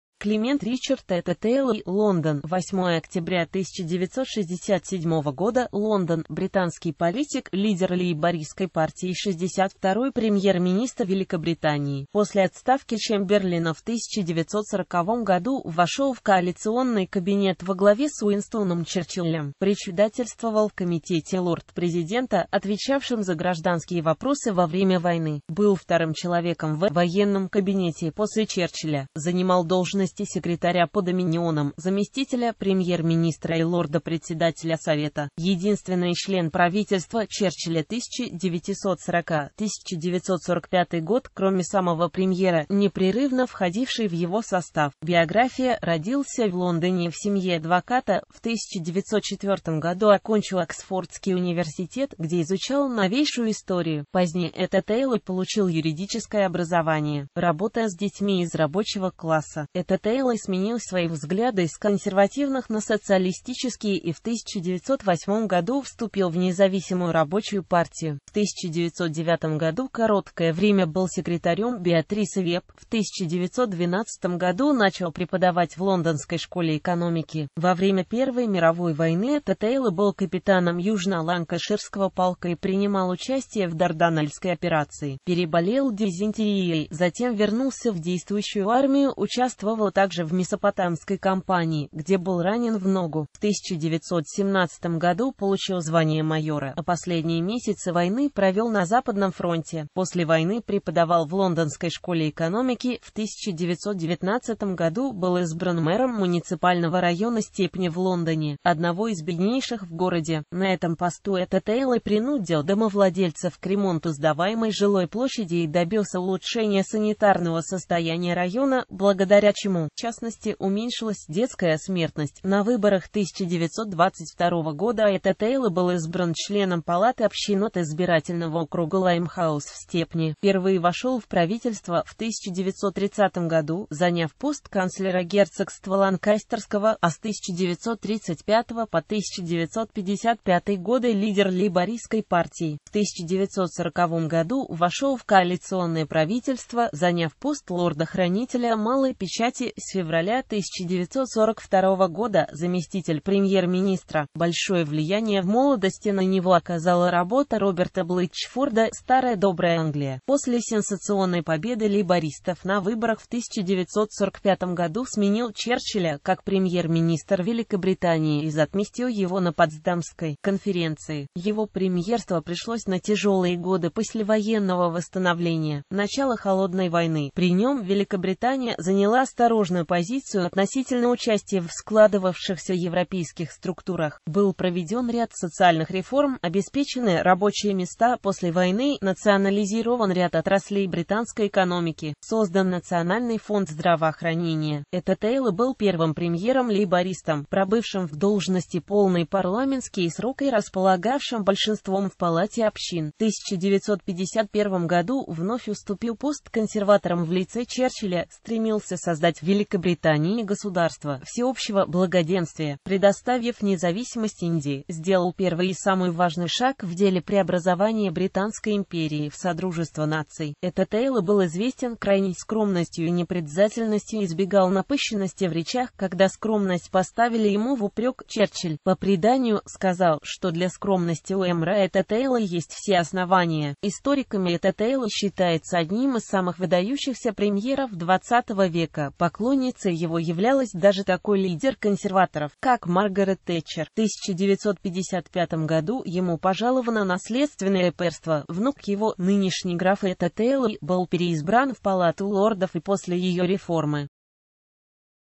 Эттли, Клемент
Клемент Ричард Эттли , Лондон, — 8 октября 1967, Лондон) — британский политик, лидер Лейбористской партии и 62-й премьер-министр Великобритании.После отставки Чемберлена в 1940 г.вошёл в коалиционный кабинет во главе с Уинстоном Черчиллем.Председательствовал в Комитете Лорд-президента, отвечавшем за гражданские вопросы во время войны, был вторым человеком в «Военном кабинете» после Черчилля.